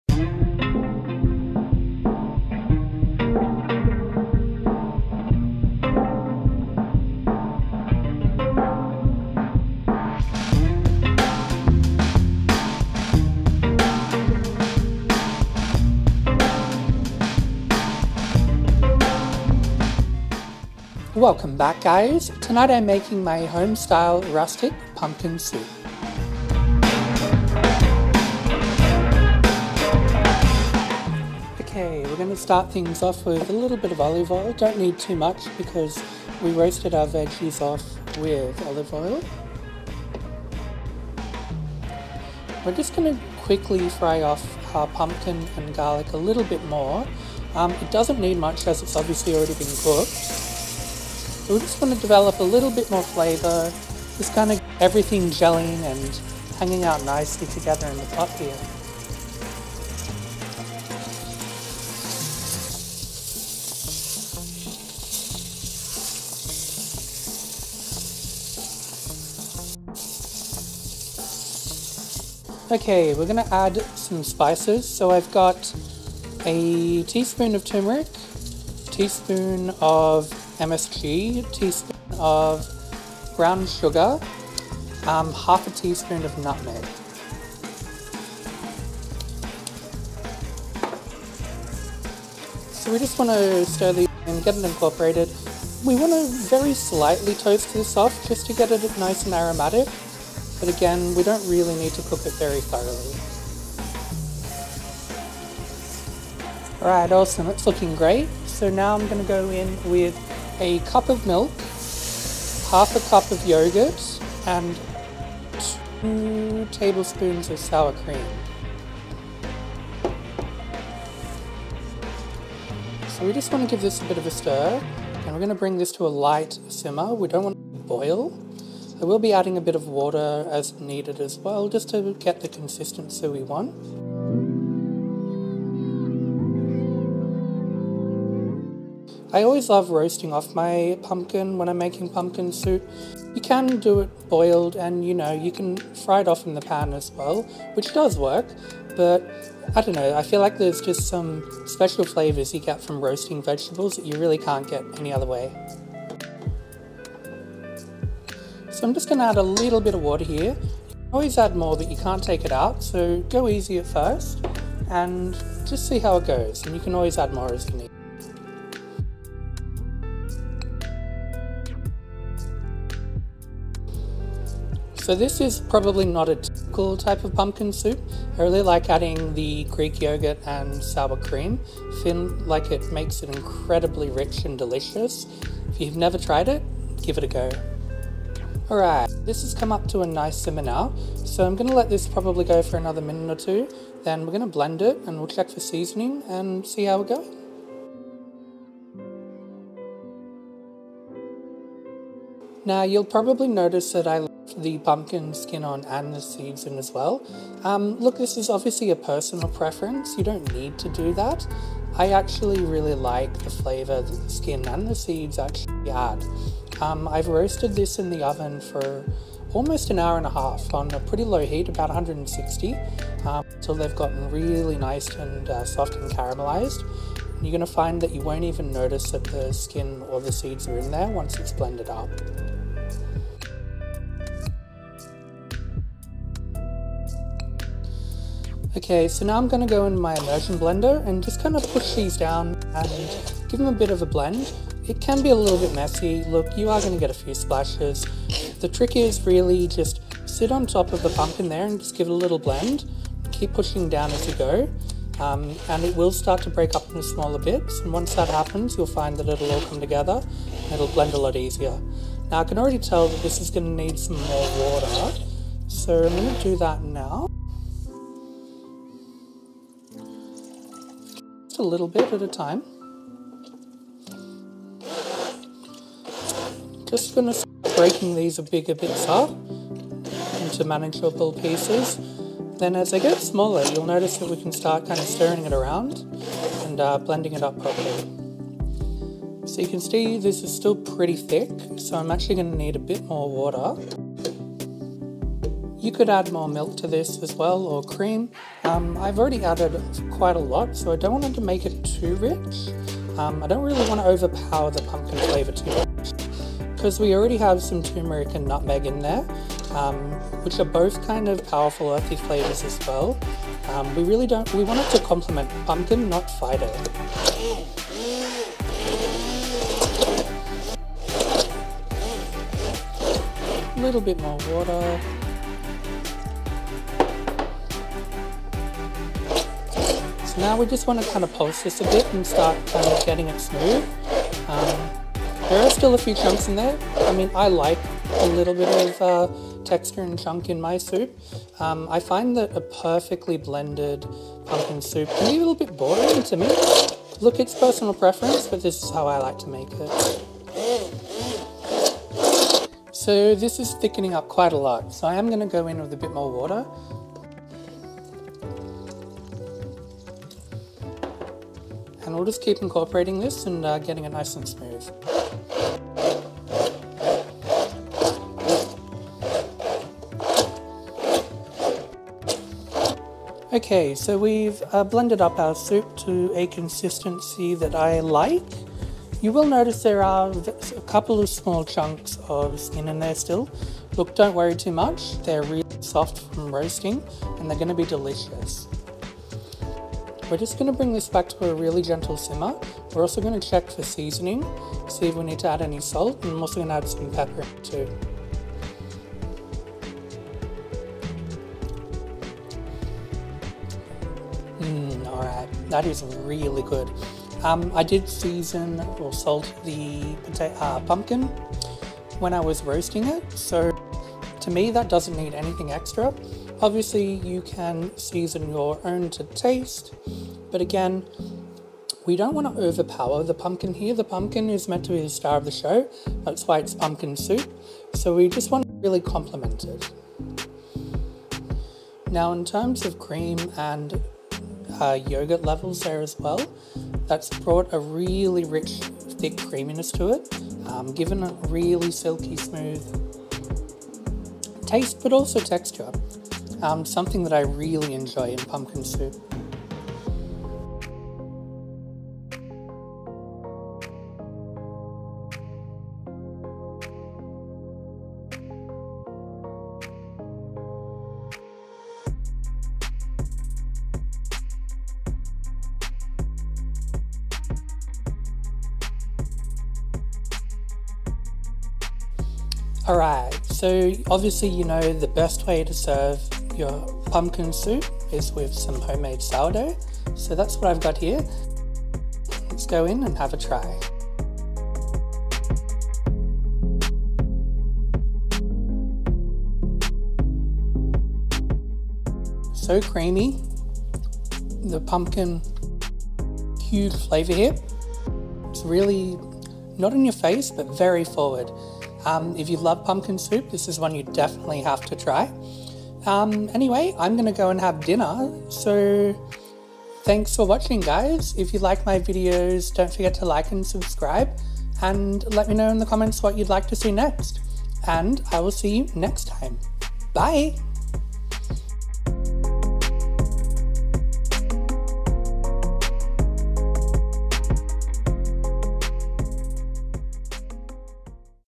Pumpkin Soup Sends You For a Loop 🥣
What's better than pumpkin soup you might ask? Well i'll tell ya, it's rustic homestyle butternut pumpkin soup. Skin on, because smooth soup is for cowards.